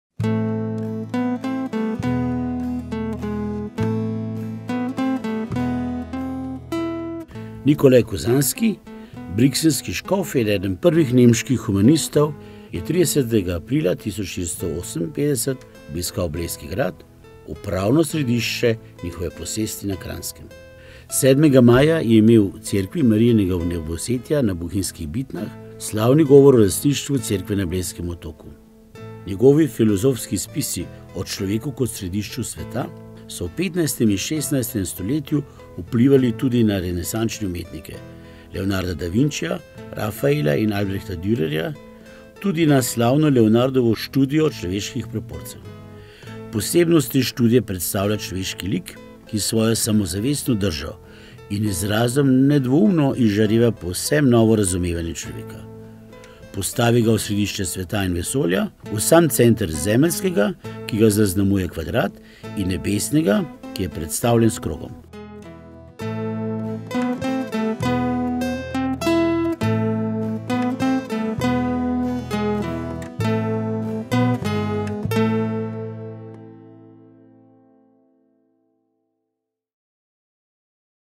Napovednik: Vpliv Kuzanskega na renesanso, predavanja o Medičejcih na Bledu
Muzejsko društvo Bled in Zavod za kulturo Bled organizirata od 5. februarja do 26. marca osem predavanj o Medičejcih. Izvedeli bomo, kdo so bili Medičejci in kakšna je njihova neverjetna zgodba, zgodba o začetku renesanse in humanizma. Spoznali bomo, kako sta se florentinska renesansa in humanizem širila po Evropi in vplivala tudi na naše kraje. Predaval bo poznavalec njihove zgodovine Miloš Klemen Mahorčič.

Predavanja na temo Medičejci:

5. feb.: Firence in Medičejci
12. feb.: Rojstvo renesanse (Cosimo Starejši)
19. feb.: Družinska politika (Piero in Lorenzo de Medici)
26. feb.: Medičejski papeži (Leon X., Klemen VII., Pij IV. in Leon XI.)
5. mar.: Od meščana do vladarja (Alessandro I. in Cosimo I.)
12. mar.: Medičejki na prestolu Francije (Katarina in Marija Medičejska)
19. mar.: Obdobje ambicij (Francesco I., Ferdinand I. in Cosimo II.)
26. mar.: Zaton (Ferdinand II., Cosimo III. in Giangastogne I.)

Brezplačna predavanja bodo vsak torek v Festivalni dvorani na Bledu z začetkom ob 19. uri. Vabljeni vsi ljubitelji zgodovine in kulture.